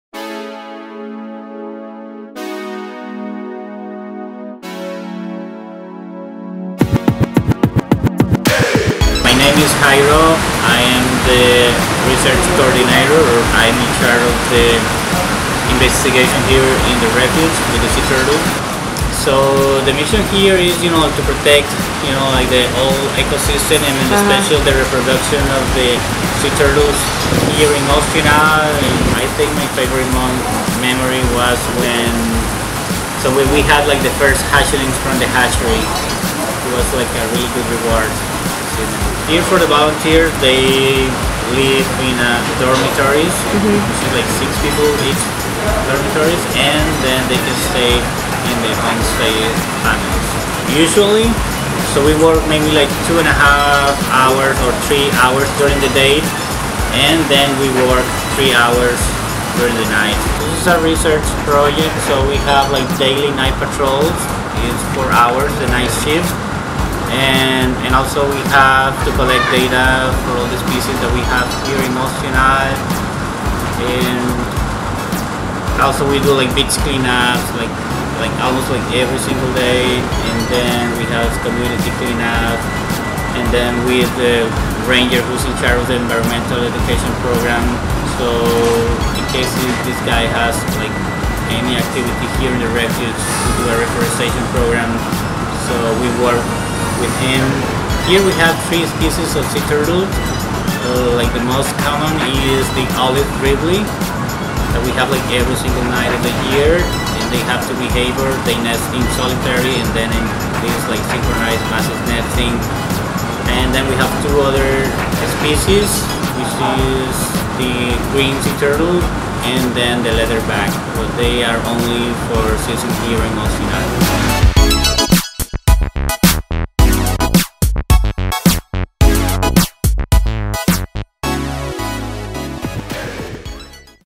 Volunteer with Sea Turtles in Costa Rica - Global Work & Travel
Love the ocean and ready for an adventure? Make a difference to the lives of these majestic creatures and explore beautiful Costa Rica at the same time on our Sea Turtle Conservation Volunteer Trip with Global Work & Travel

See the world in a meaningful way